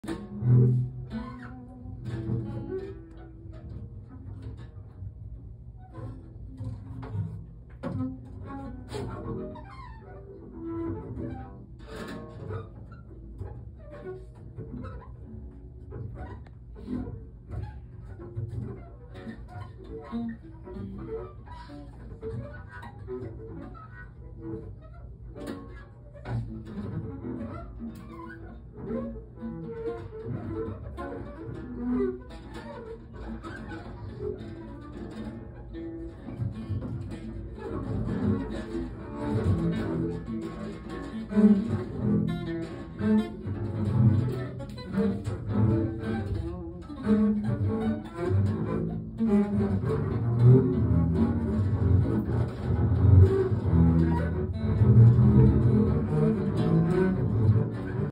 Nat Baldwin & Jason Gibbs double bass and guitar at John Doe Jr Johndoejunior.com 9/8/22
Liz Tonne sings at John Doe Jr johndoejunior.com as part of Nat Baldwin’s Triptick Tapes series